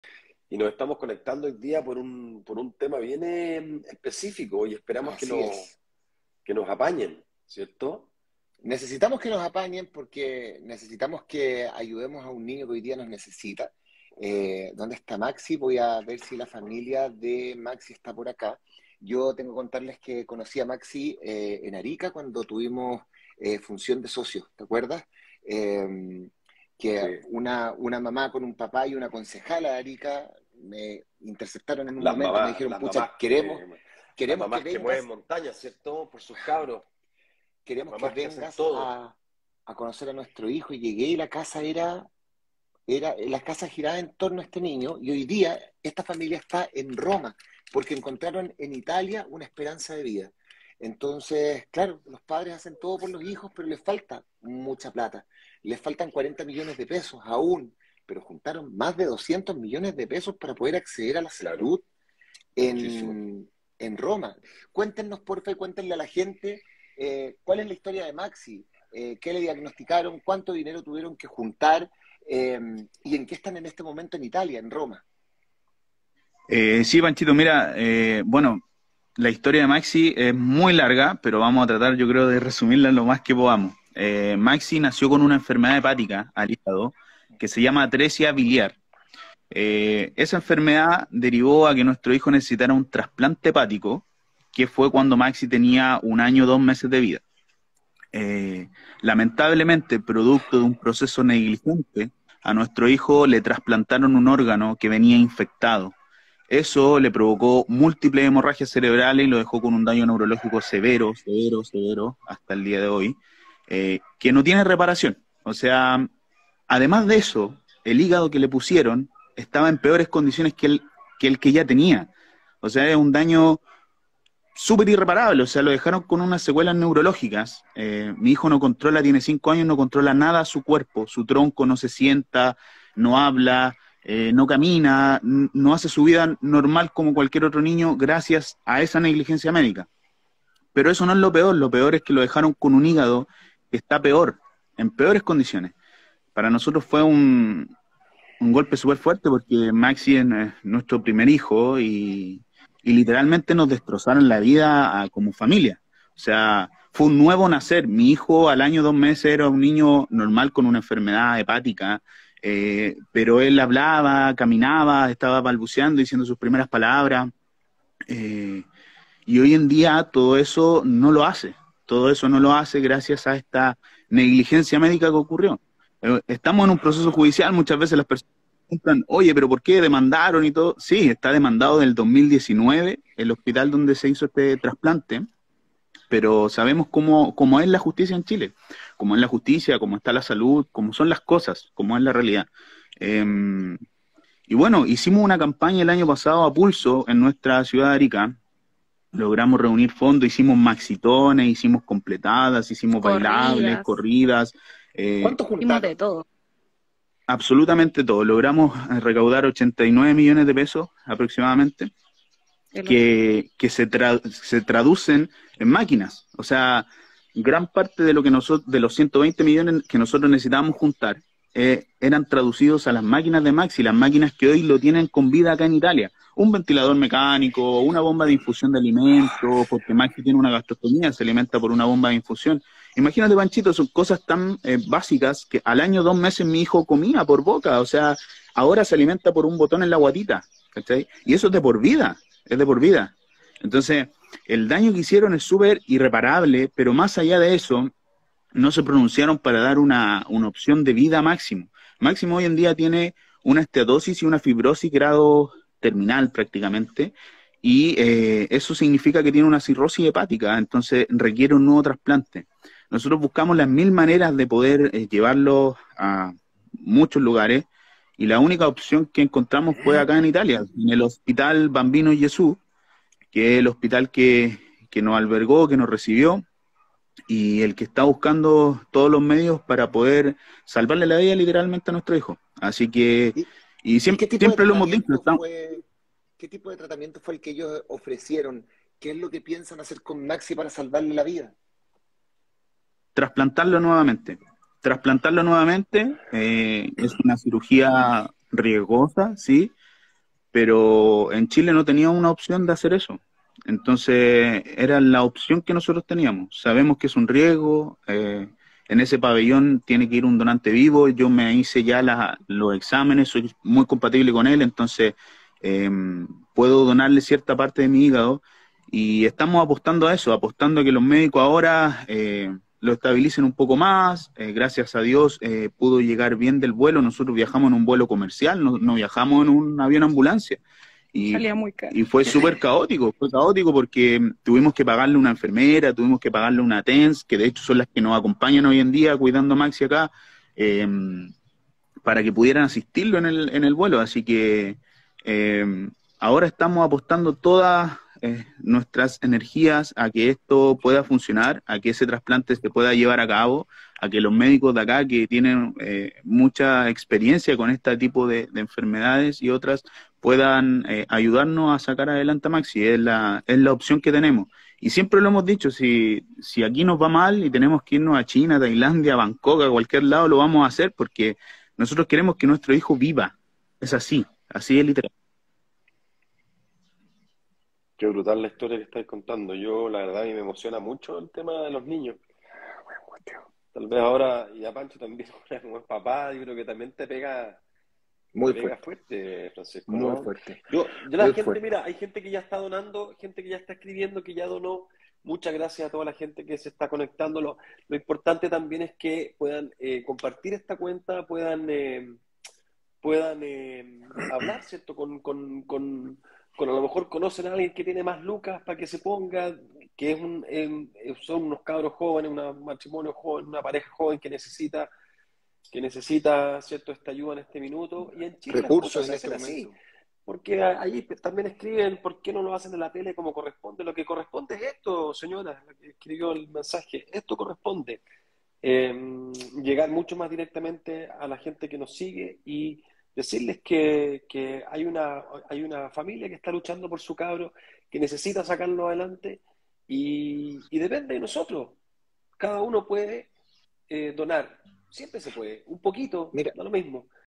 PANCHO SAAVEDRA Y JORGE ZABALETA SE UNEN EN AYUDA DE NIÑO QUE SUFRIÓ NEGLIGENCIA MÉDICA
¿Cómo donar a Máximo y su familia?
Máximo y su familia ahora están en Roma, porque encontraron en Italia una esperanza de vida. Haciendo muchas actividades en Arica, lograron reunir 89 millones de pesos que, según su padre, se traducen en máquinas que los mantienen con vida.

Para donar, toda la información está disponible en su página web y también su cuenta de Instagram.

Más detalles en la nota en TiempoX.cl